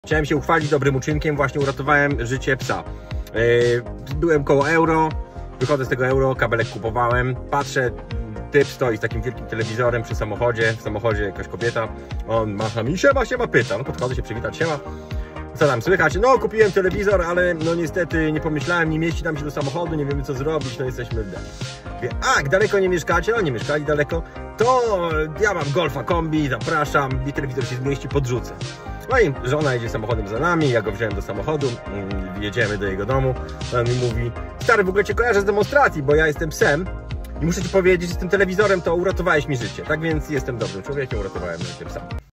Jaok uratował życie psa
Jaok opowiada o przygodzie która go spotkała.